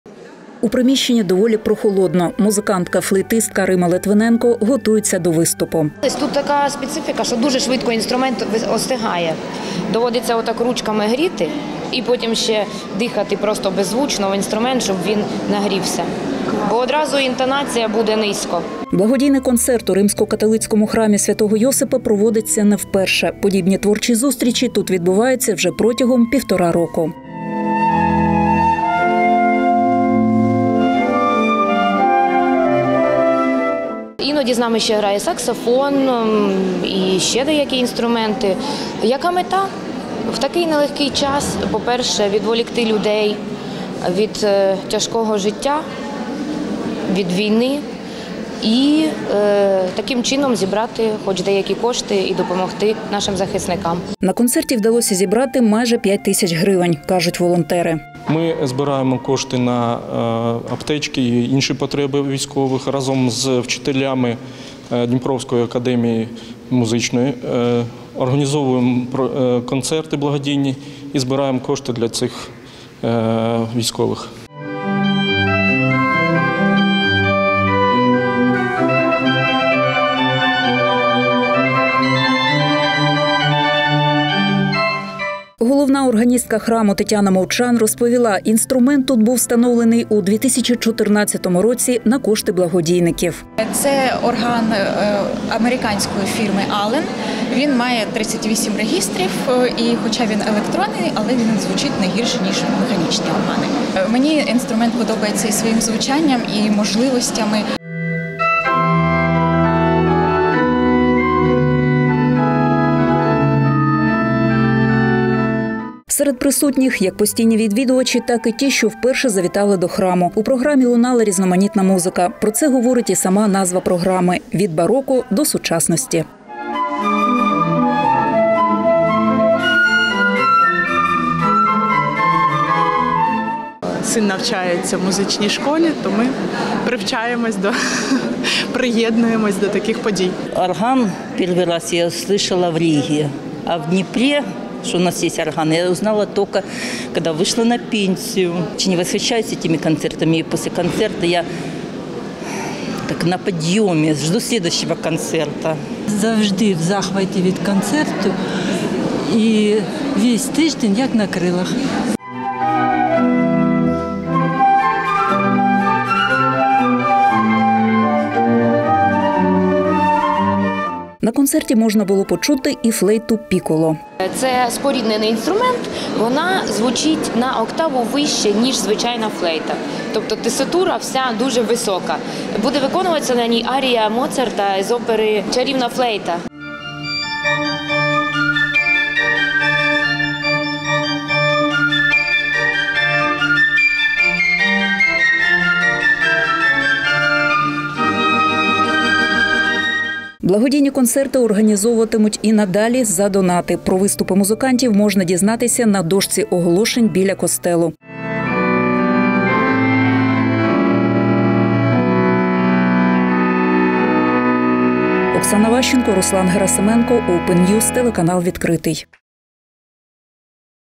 Благодійний концерт в католицькому храмі Дніпра. Зібрали кошти на аптечки для захисників
Орган та флейта - у програмі «Від бароко до сучасності». У Дніпрі на благодійному концерті збирали кошти на аптечки для військових. Творча зустріч пройшла в католицькому храмі. Докладніше в наступному сюжеті.